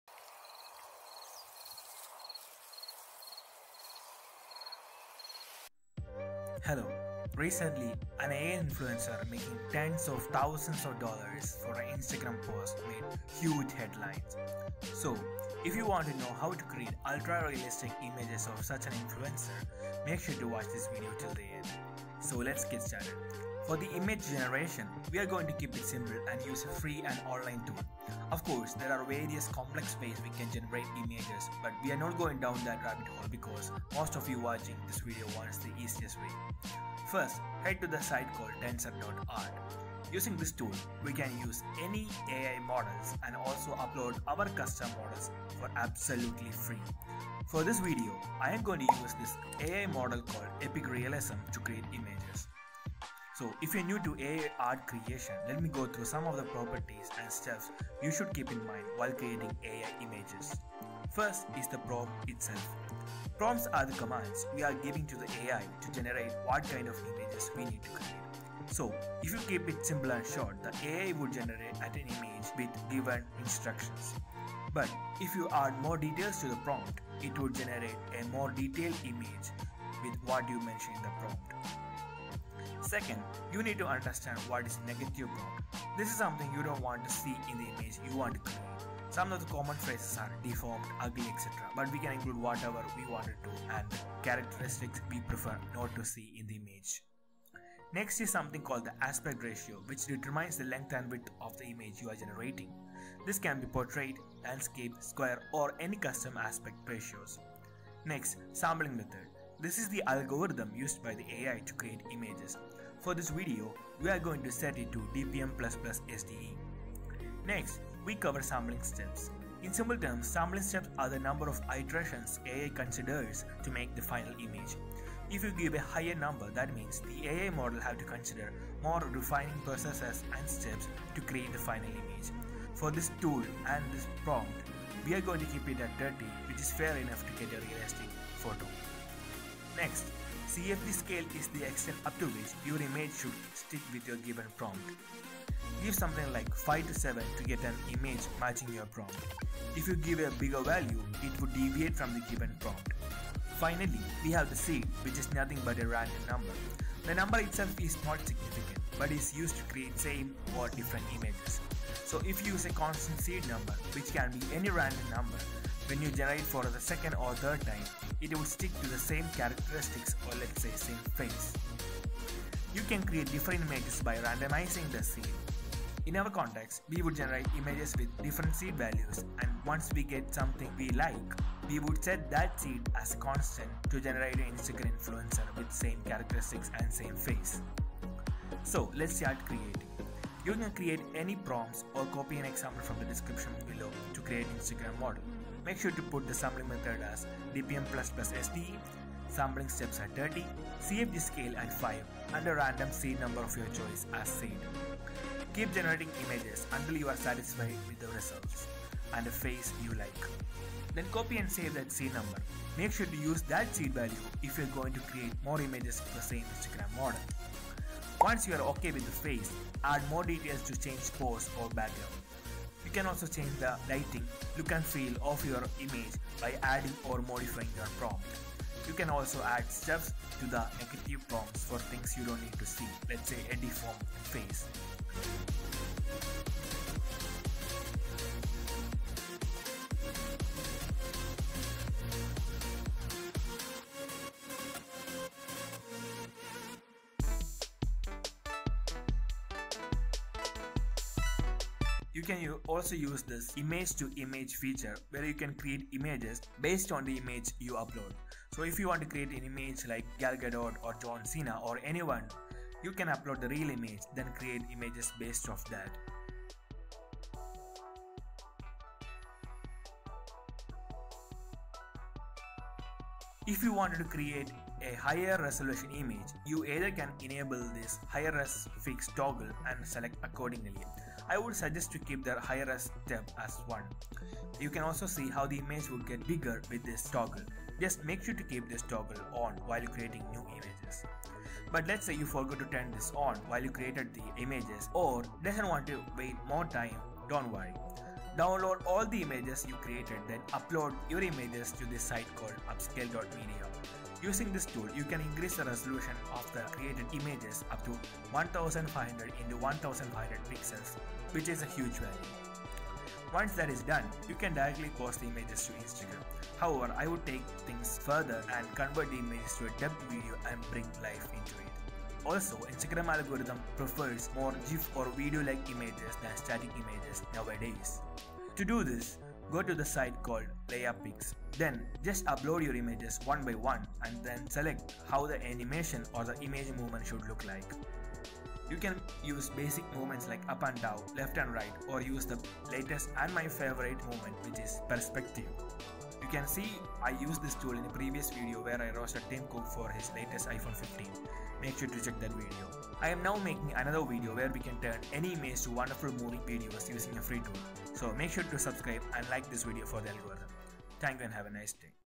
Create AI Influencer and Make $2,500/Month Using This FREE Tool
Hello. Recently an AI Influencer making tens of thousands of dollar for an Instagram post made huge headlines. So if you want to know how to create ultrarealistic images of such influencer, make sure to watch this video till the end.

For the image generation, we are going to keep it simple and use a free and online tool. Of course, there are various complex ways we can generate images but we are not going down that rabbit hole because most of you watching this video wants the easiest way.

*Settings*

_Prompt_
RAW Photo, DSLR, a young woman, (light smile:0.8), (smile:0.5), wearing relaxed shirt and trousers, casual clothes, (looking at viewer), focused, (modern and cozy office space), design agency office, spacious and open office, Scandinavian design space BREAK detailed, natural light

_Negative Prompt_ : asian, chinese, busty, (epicnegative:0.9)

_Sampling Method_ :DPM++ SDE
_Sampling Steps_ : 30
_CFG Scale_ : 5
_Seed_ : A random number you prefer

Note: Forgot to mention, if you are using the inbuild Upscaler, you need to set the "Upscale By" to atleast 1.5x. I have demoed it setting at 1x which would remain at the same scale of original creation.